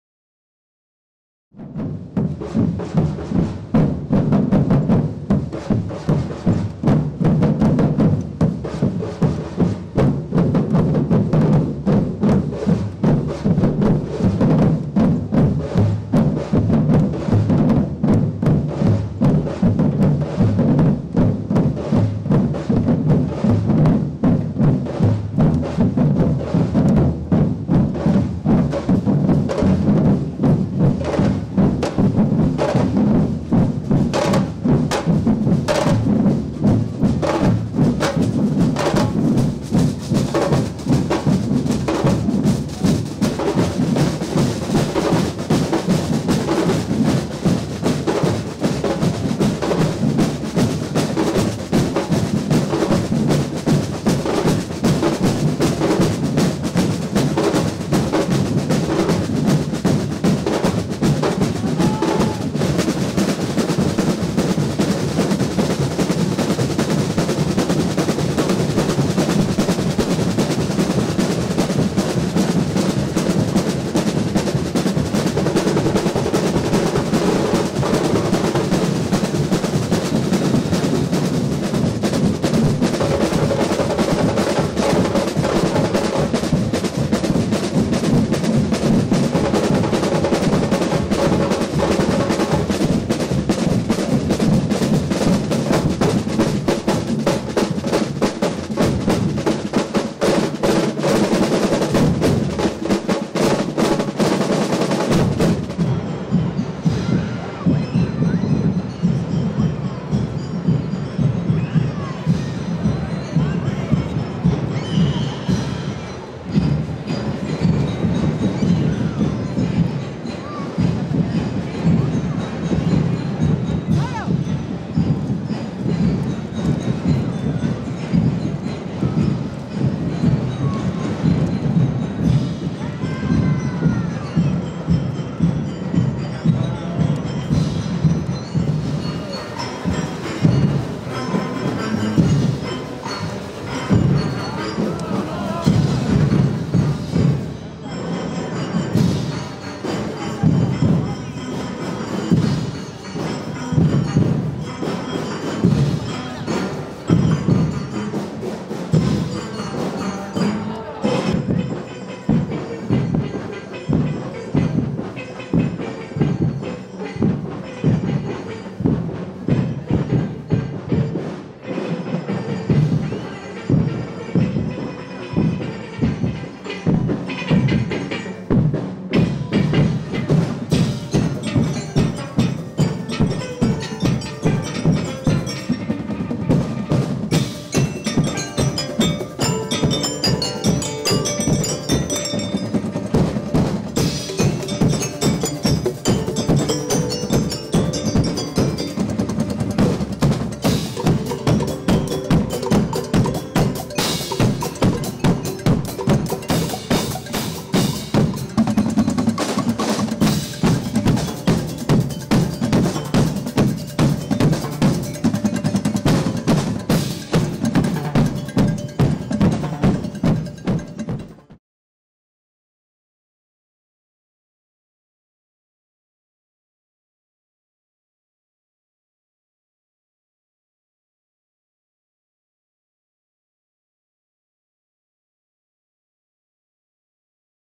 Comparsas Batalá y Balumba Desfile Sardina 2023
Más comparsas desfilando en el Entierro de la Sardina 2023.